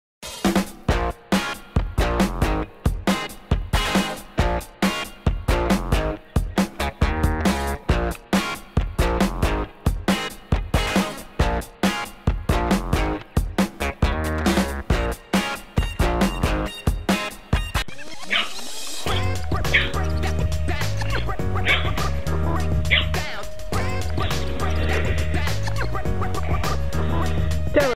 Dancing French Bulldog
In this funny dog video, watch as this cute french bulldog grooves out to some sweet tunes. This little puppy sure knows how to shake it!

Petsami delivers your daily fix of LOL pet clips and premium original shows for animal lovers of all ages.